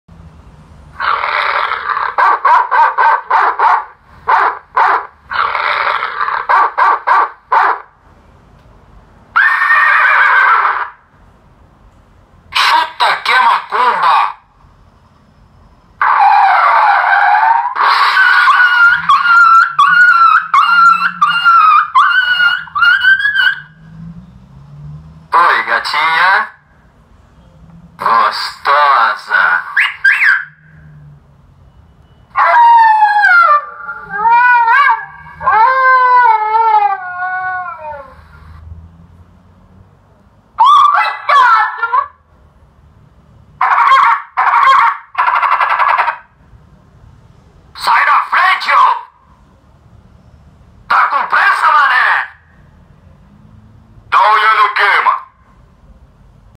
Buzina eletrônica 12 toques “Sai da Frente”
Com essa buzina eletronica vocês irá se divertir por onde passar !!!

Cavalo, Freada+batida de carro+cachorro gritando, Sai da Frente, Oh Coitado, Tá com Pressa Mané !!, Tá Olhando O Quê !!!, Risada, Chuta que é Macumba, Cão Bravo, Oi Gatinha, Lobo uivando, Gostosa+assobio (fiu-fiu)

Ótimo custo beneficio, facíl de instalar, controle simples e discreto.